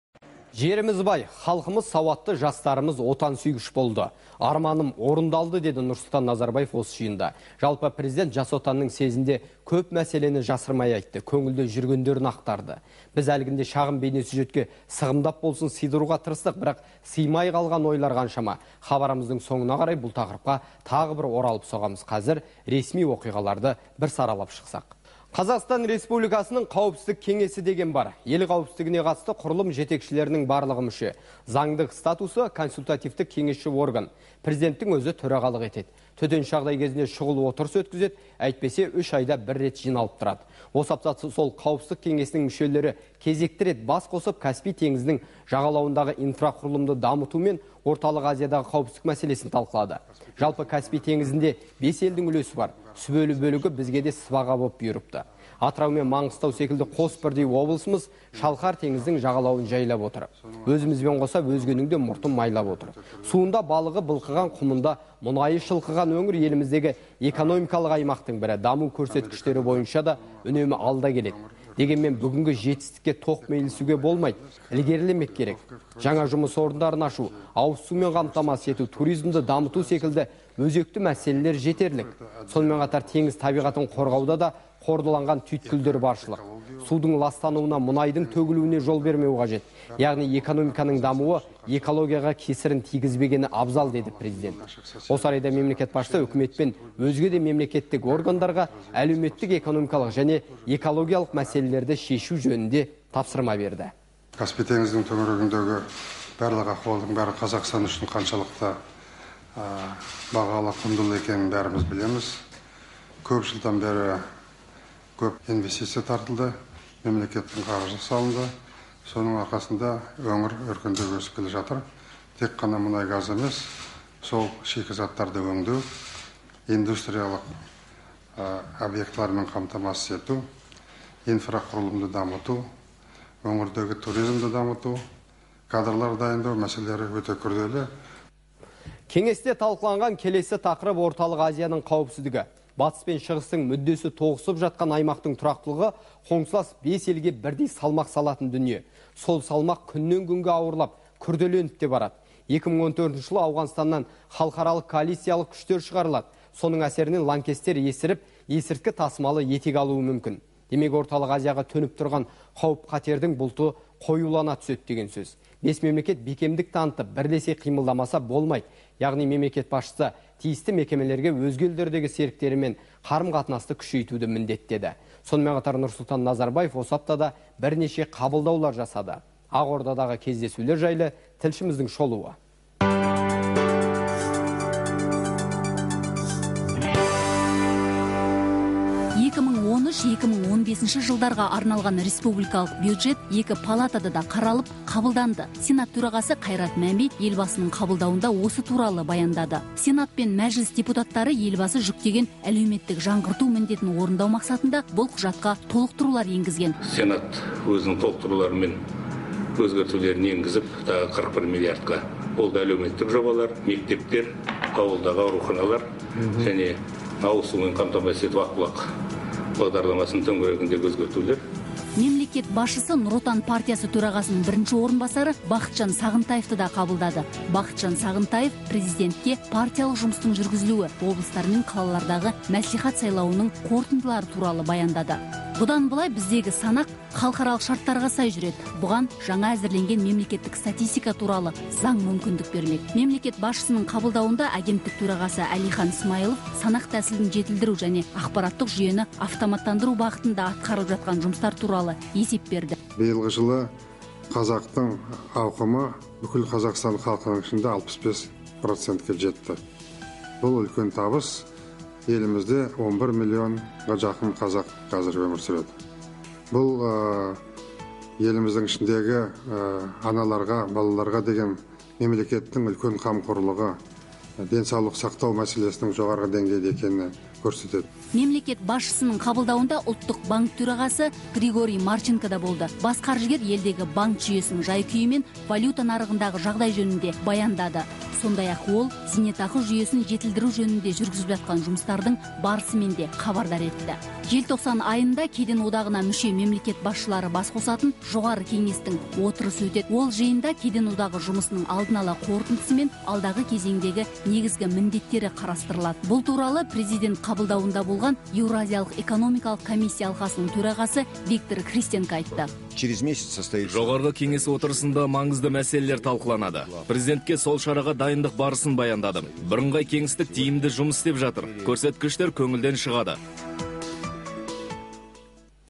Ақордадағы кездесулер
Мемлекет басшысы Үкімет пен өзге де мемлекеттік органдарға әлеуметтік-экономикалық және экологиялық мәлелелерді шешу жөнінде тапсырма берді.